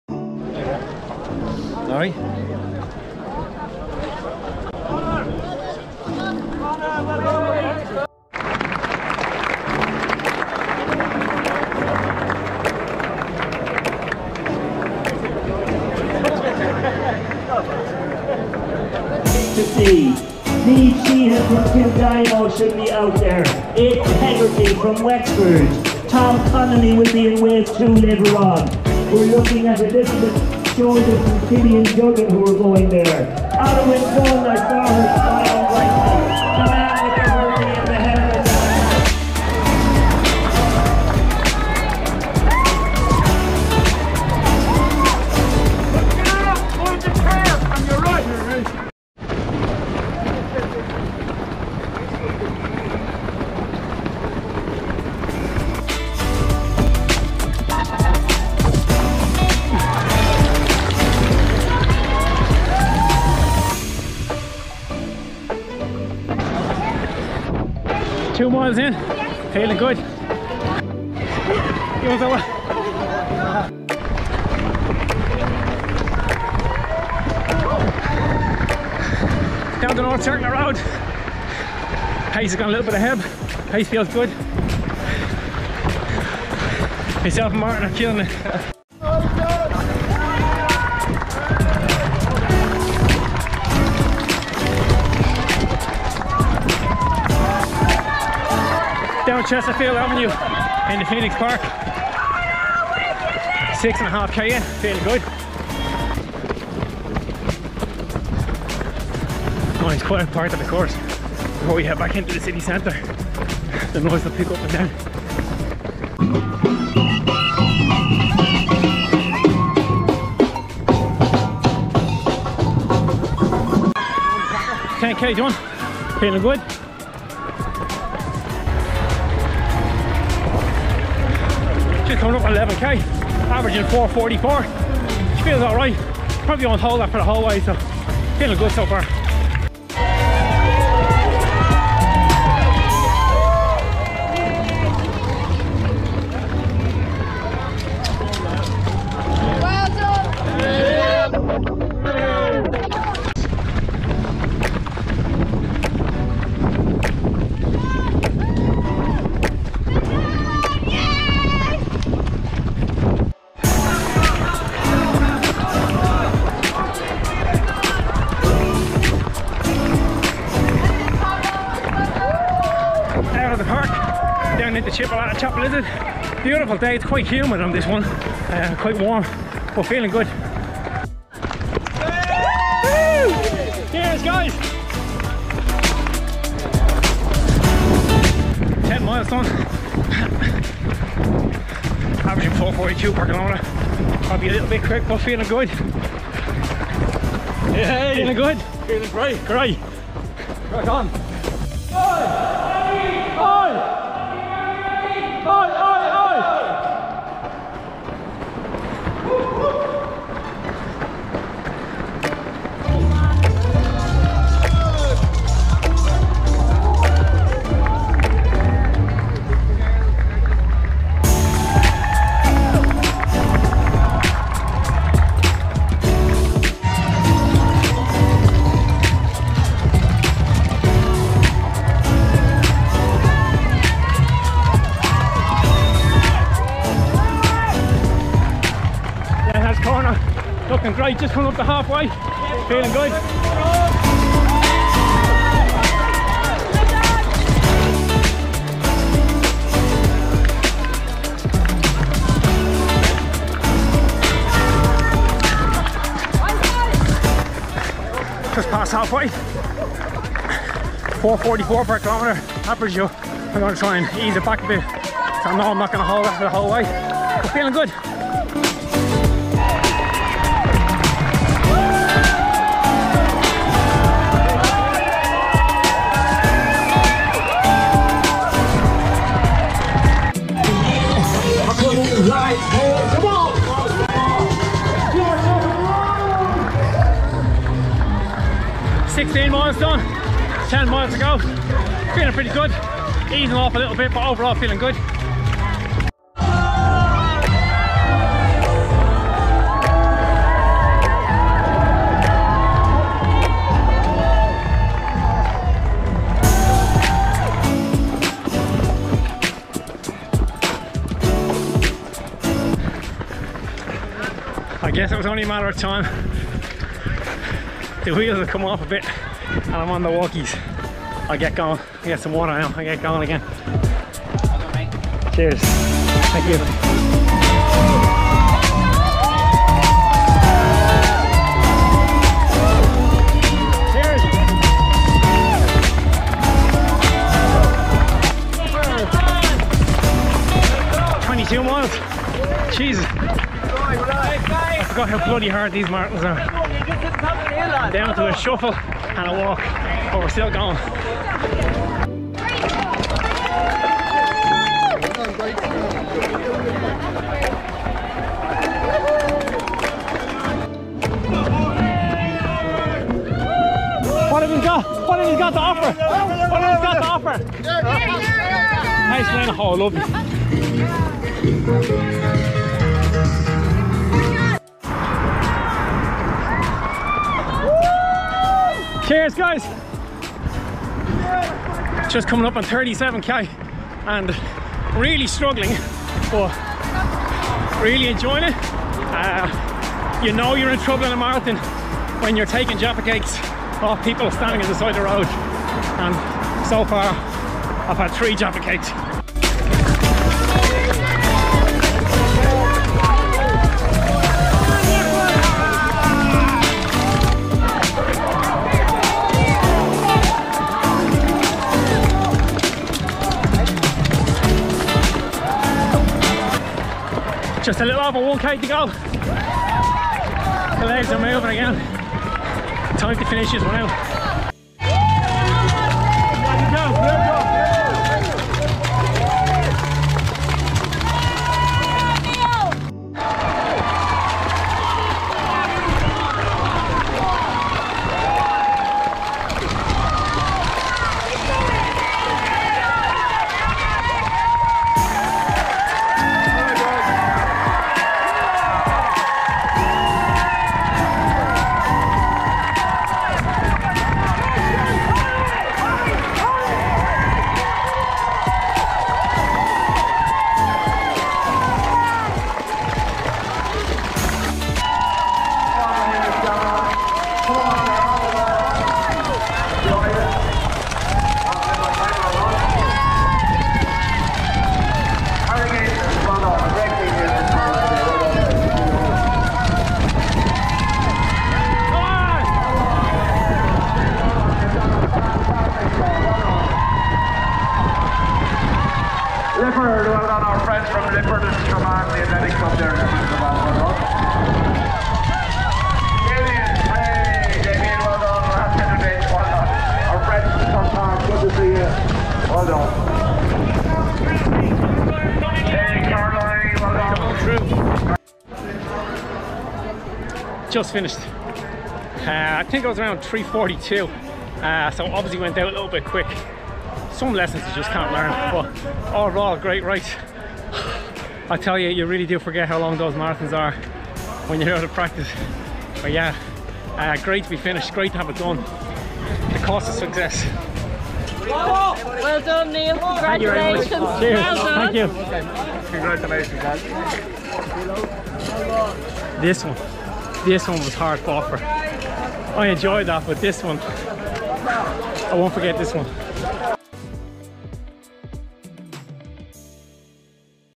Dublin City Marathon 2022
This is my whole marathon in 9mins. I'm really happy with having finished. There was a good case for me not doing it at all this year but I'm so pleased that I did. I believe this is the greatest race I've ever done. Every year the support is spectacular. Thank you!